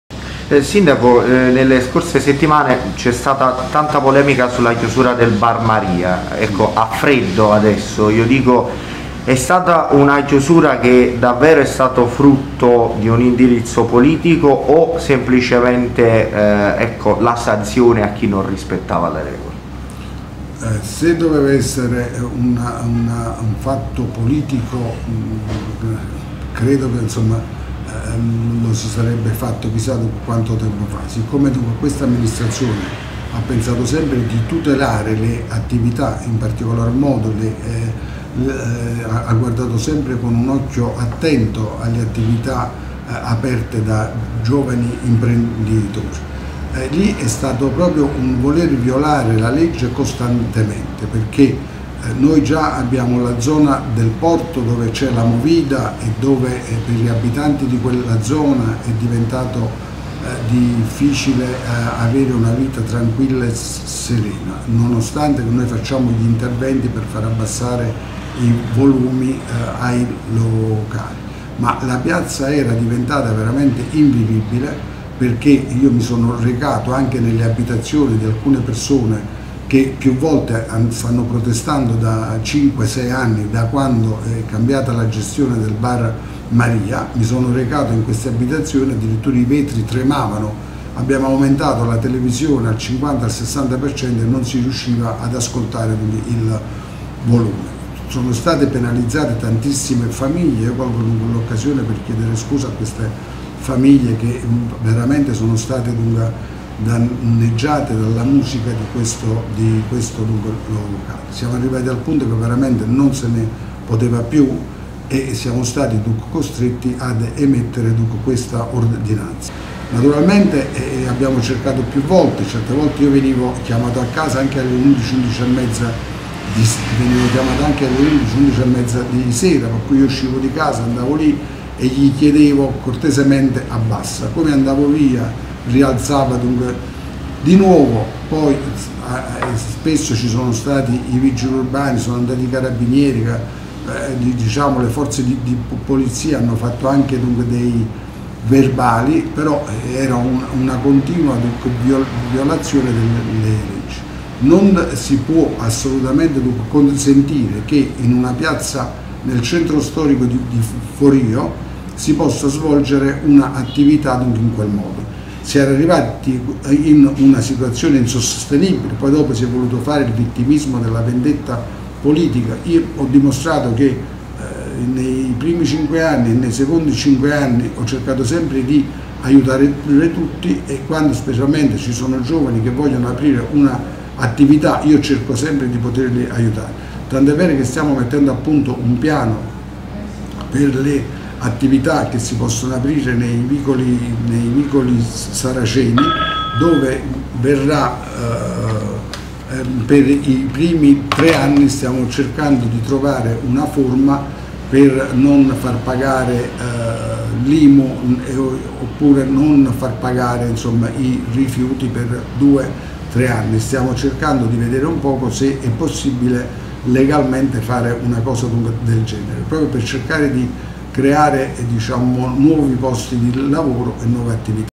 DEL DEO SULLA CHIUSURA DEL BAR MARIA: “IN UN CENTRO STORICO NON SI PUO’ FARE MUSICA IN QUEL MODO"
A qualche settimana dalla discussa ordinanza che di fatto chiuse il Bar Maria nel centro di Forio, abbiamo chiesto al sindaco Francesco Del Deo le motivazioni che hanno portato il dirigente competente ad adottare un provvedimento così duro. Il primo cittadino foriano ha raccontato la sua versione, sottolineando come da anni andava avanti una situazione ingestibile per tanti cittadini della zona: “Sono vicino ai giovani e alle imprese, ma questo non è modo di fare turismo in un centro storico”, ha dichiarato Del Deo.